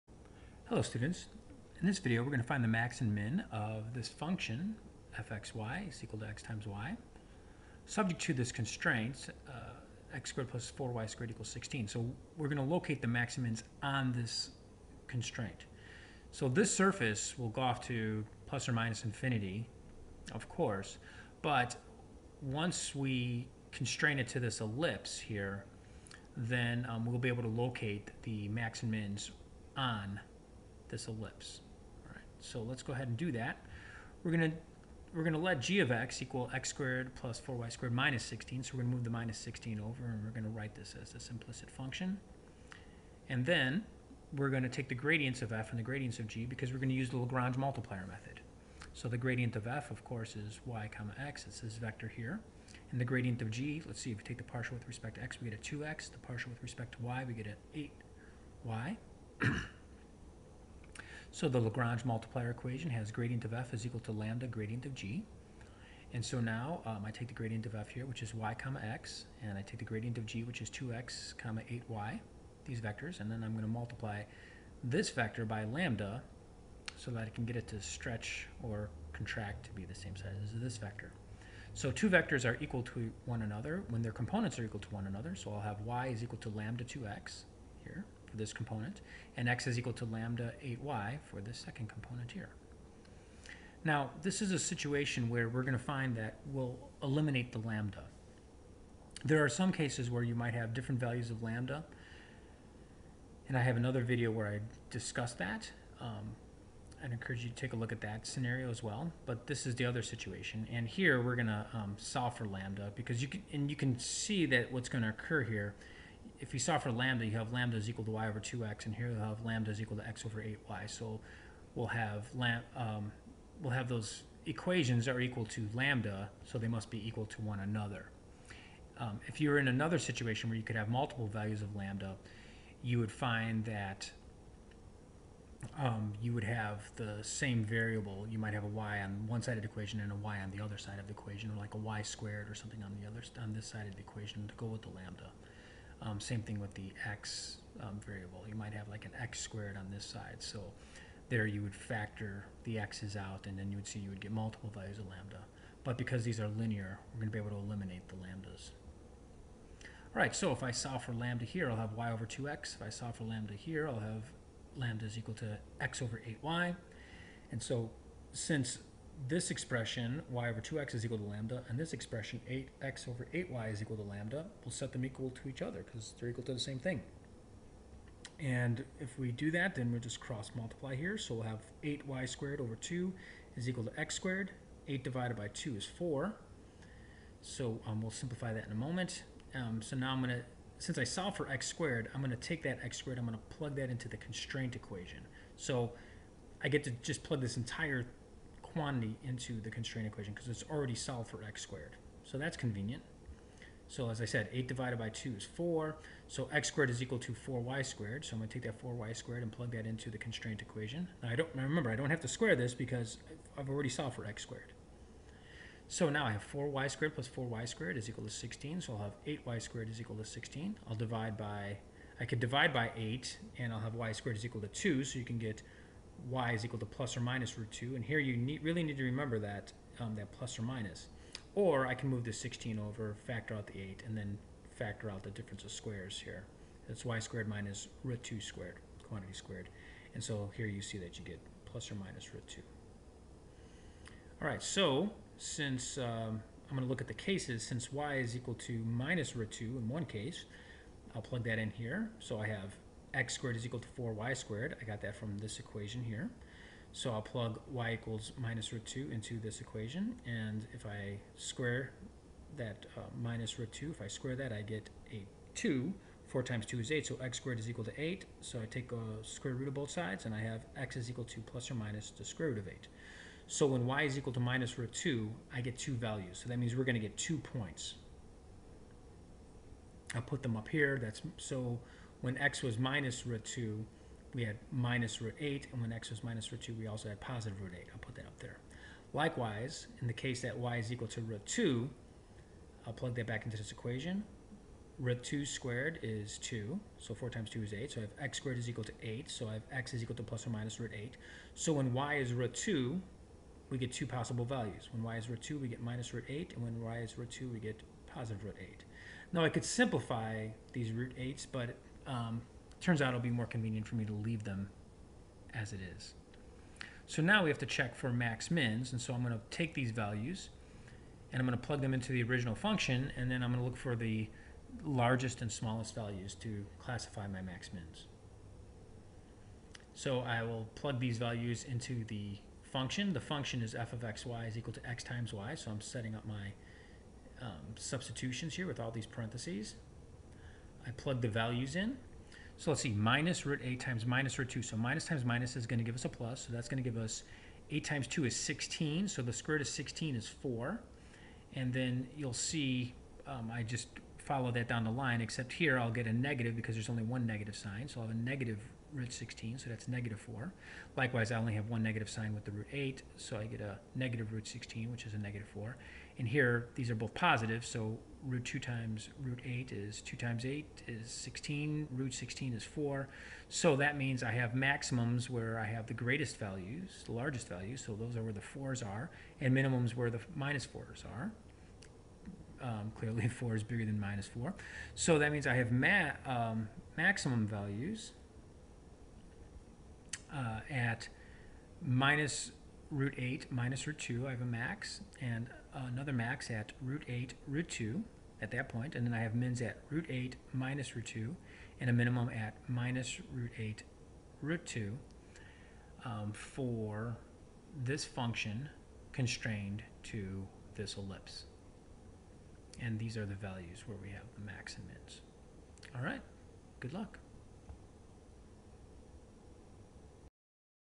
Lagrange Multiplier Method - Eliminate Lambda
Find the maximum and minimum of f(x,y) = xy constrained to the ellipse x^2 + 4y^2 = 16.